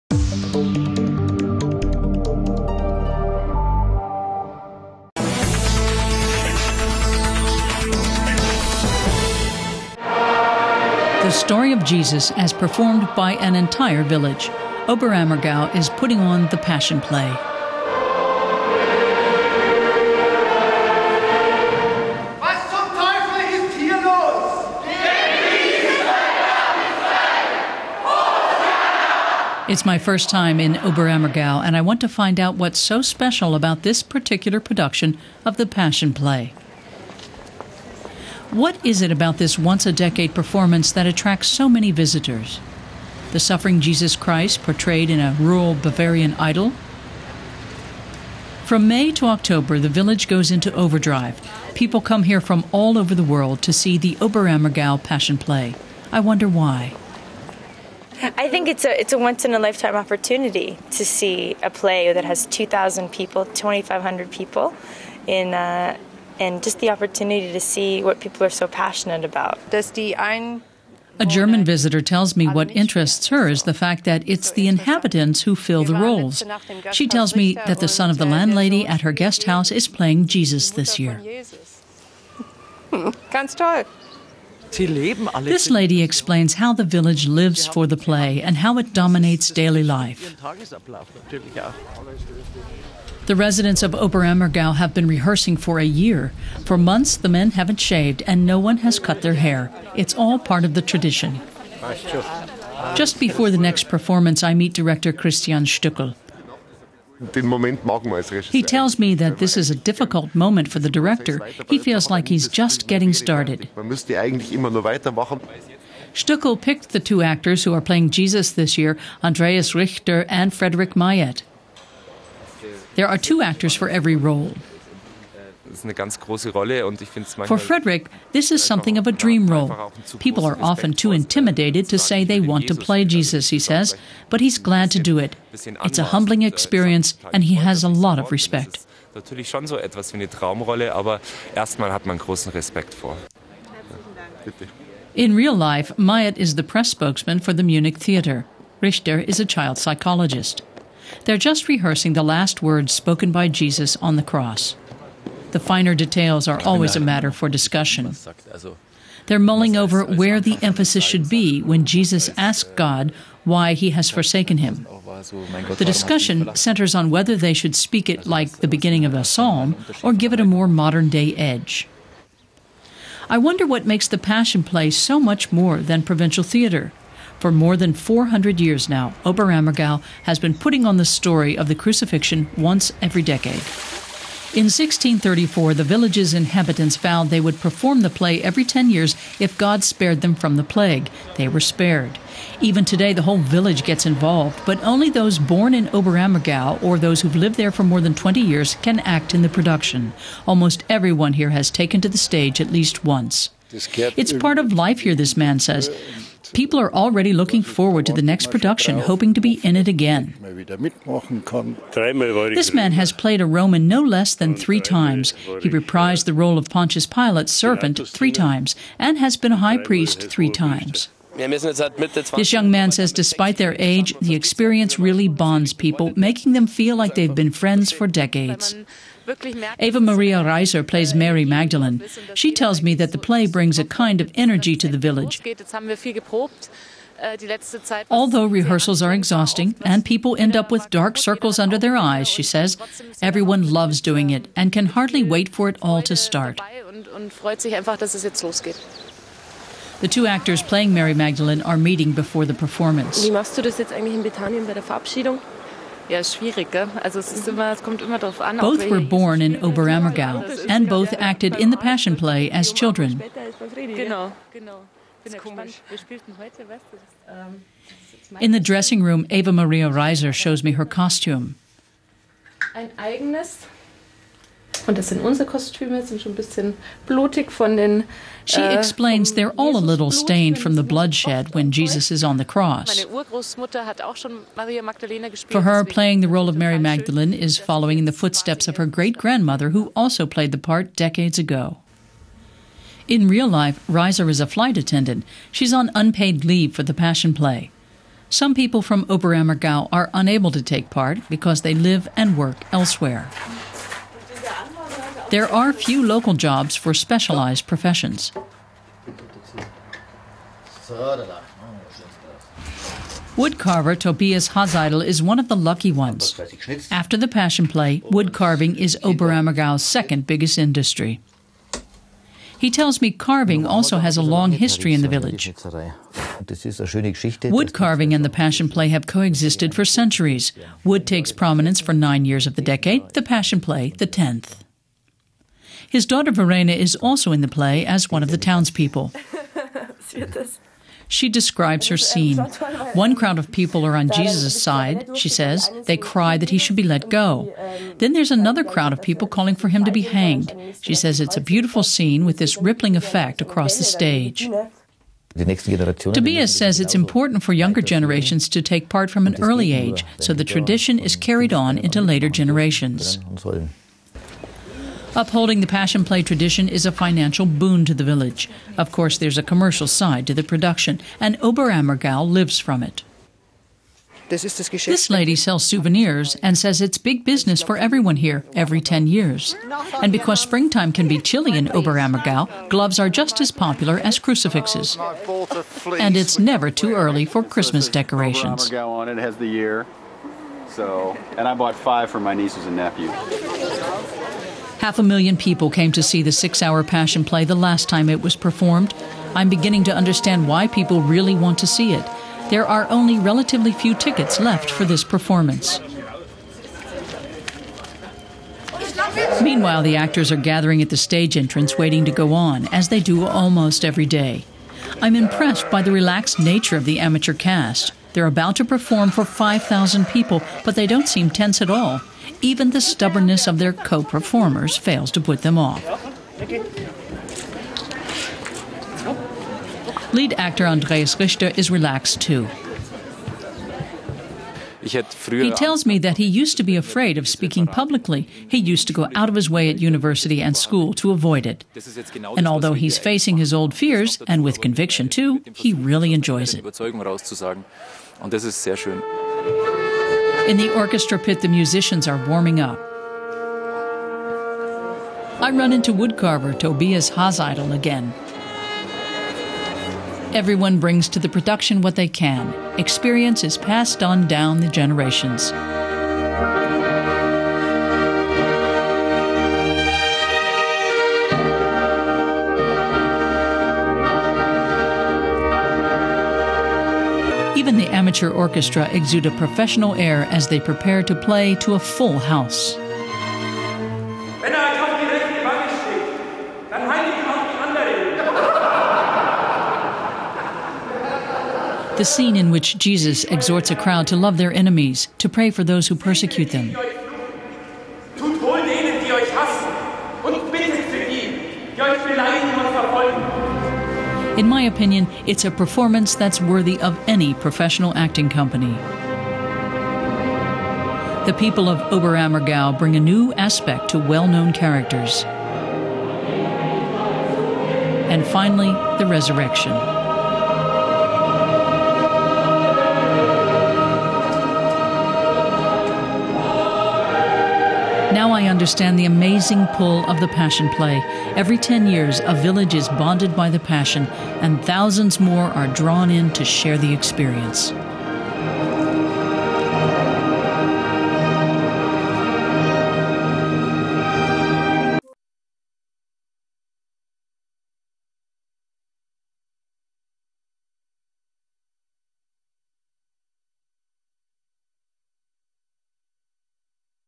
The Oberammergau Passion Play
Elena Ern goes behind the scenes of the Oberammergau Passion Play